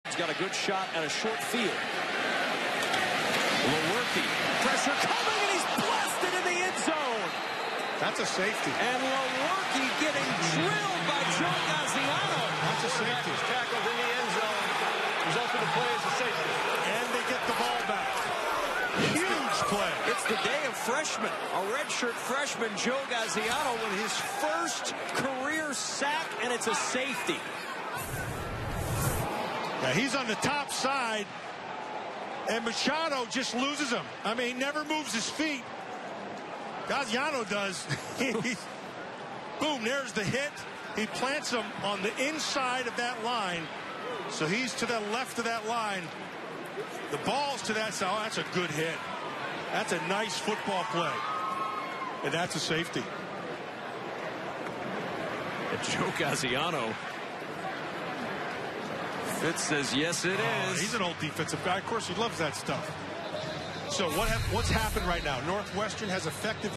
Joe Gaziano Safety vs. Michigan State
Michigan State's Brian Lewerke gets sacked by Northwestern's Joe Gaziano for a safety in the second quarter.